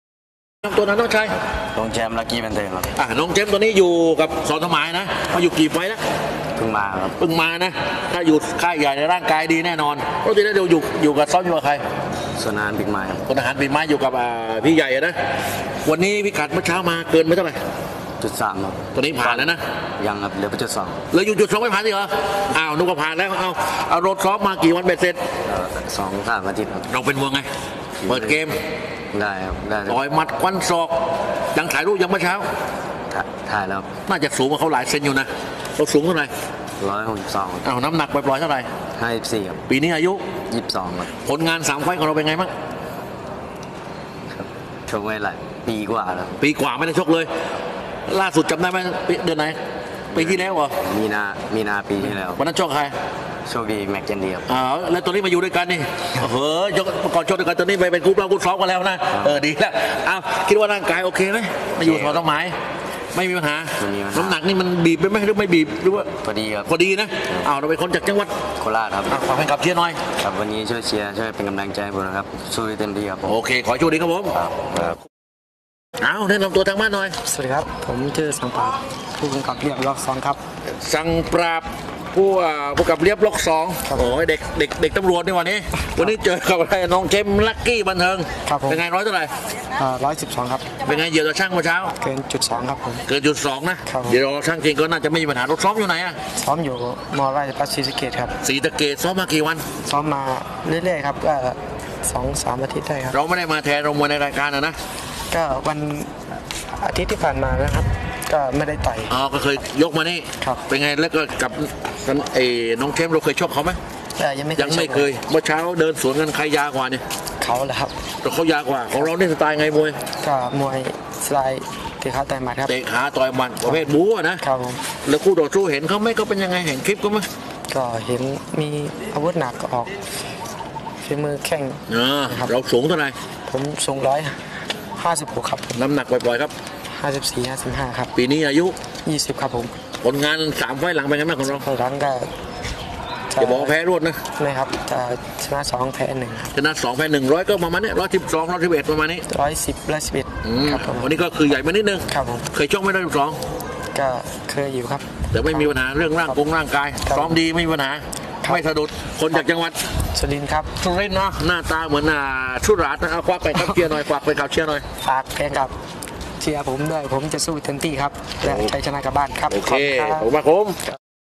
สัมภาษณ์ น้องแชมป์ VS สั่งปราบ ศึกมวยไทยพันธมิตร เวทีมวยกองทัพอากาศ ช่อง JKN18 เริ่ม 18.15 น
มวยไทยวันนี้ ชั่งก่อนชก มวยไทยพันธมิตร สัมภาษณ์ น้องแชมป์ VS สั่งปราบ ศึกมวยไทยพันธมิตร ณ เวทีมวยกองทัพอากาศ (ธูปะเตมีย์) ช่อง JKN กด 18 และ เริ่ม 18.15 น

21/08/66